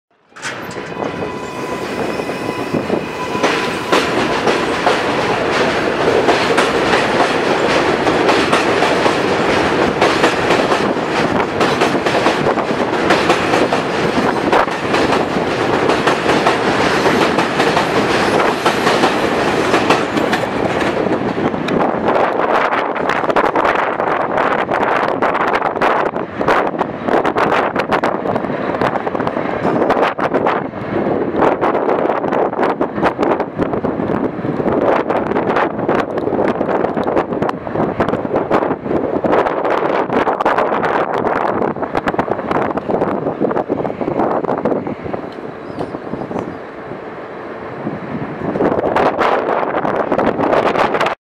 R188 7 leaving Woodside station
R188 leaving Woodside station on my trip from nyc